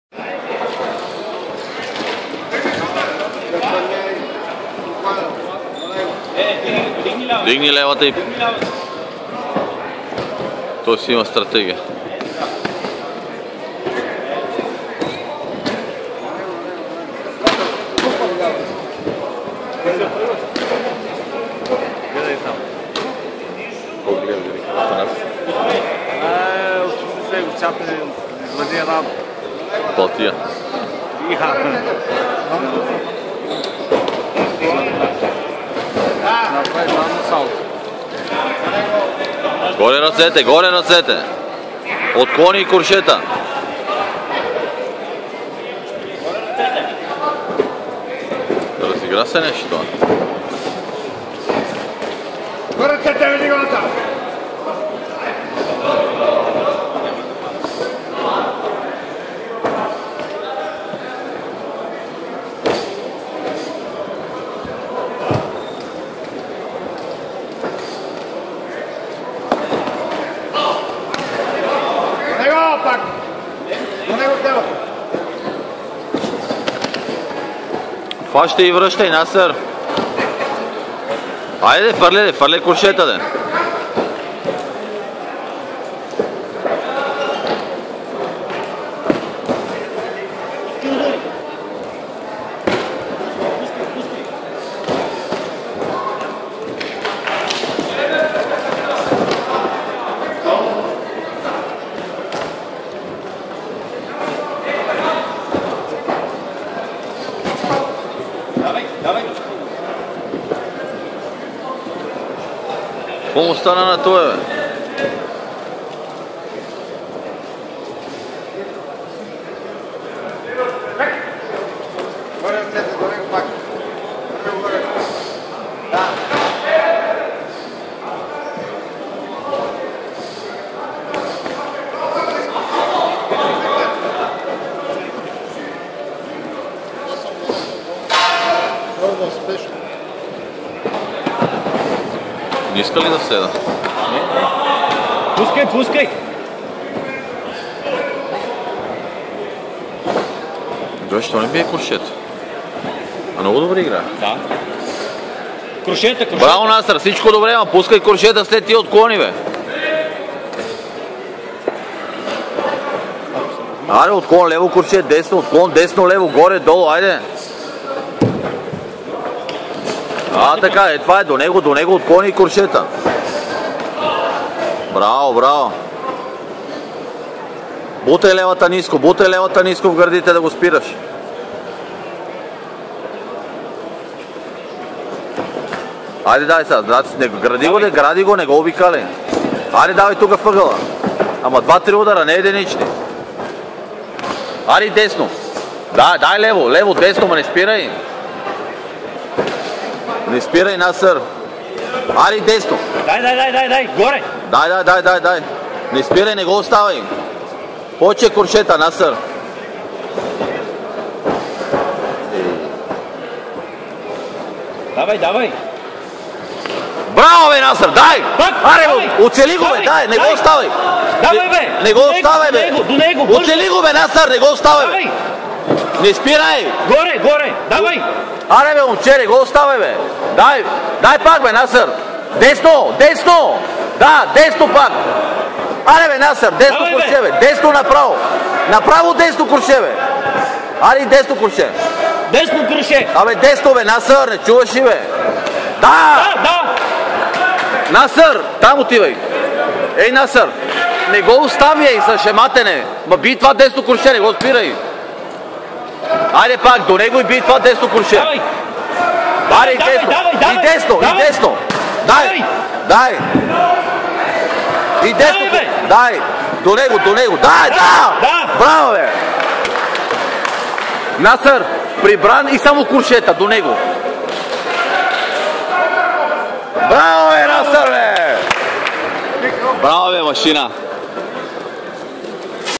Nasser Al-Ahmad VS Svetoslav Hristov - 2016 Bulgarian National Boxing Competition
Nasser Al-Ahmad (red) VS Svetoslav Hristov (blue) - 2016 Bulgarian National Boxing Competition